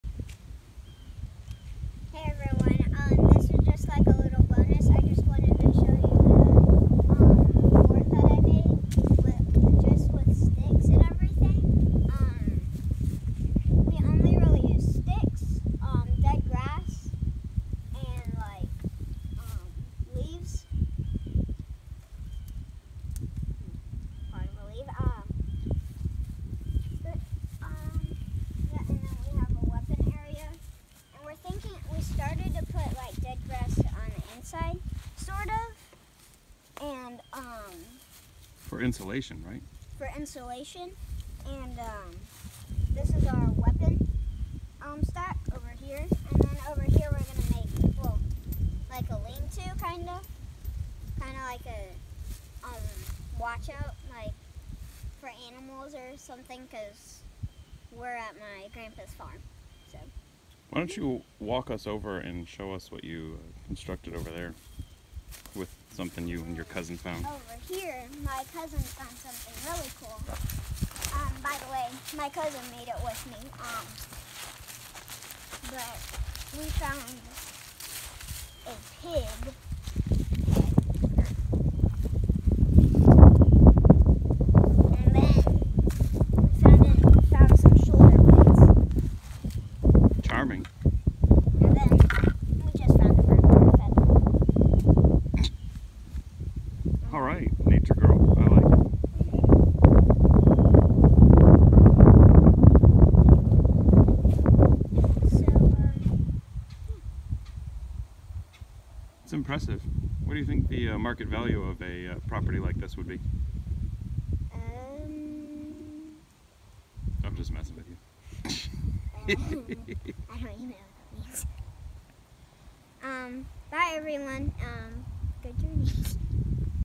Fort!
In this Bonus Bit, Hermione shows off the nifty Fort she and Cousin Freddy constructed while visiting the country, as well as some rather macabre discoveries...